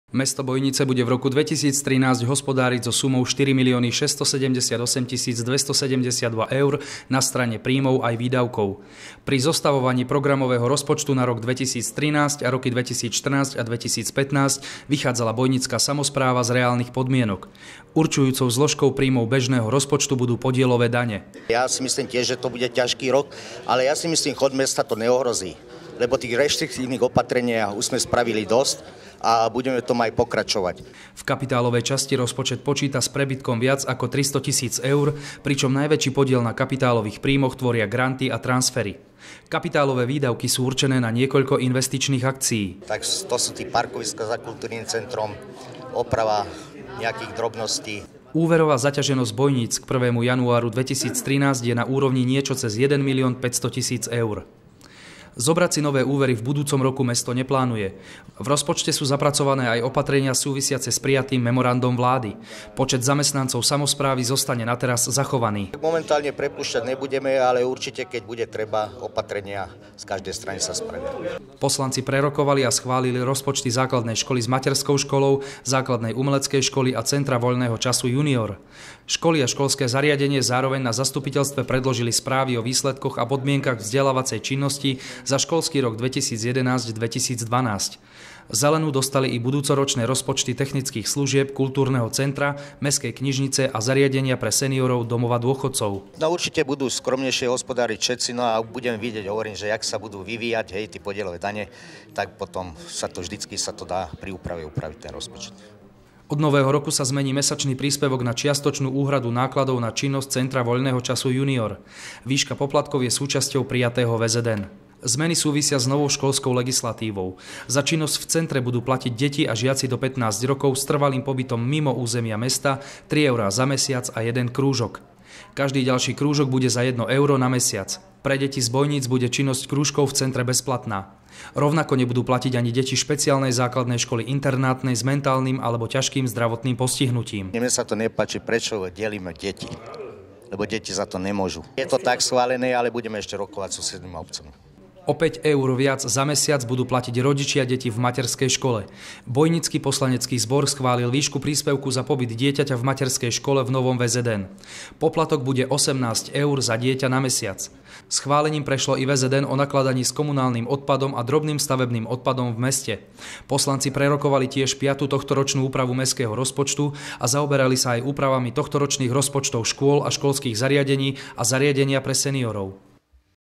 Rozpočet mesta schválený aj v Bojniciach
Rokovanie o mestskom rozpočte a rozpočtoch mestských organizácií bolo nosnou témou aj bojnického zastupiteľstva. Konalo sa 13. decembra tohto roku. Okrem iného, posledné tohtoročné zasadnutie poslaneckého zboru prinieslo aj prijatie troch nových všeobecne záväzných nariadení.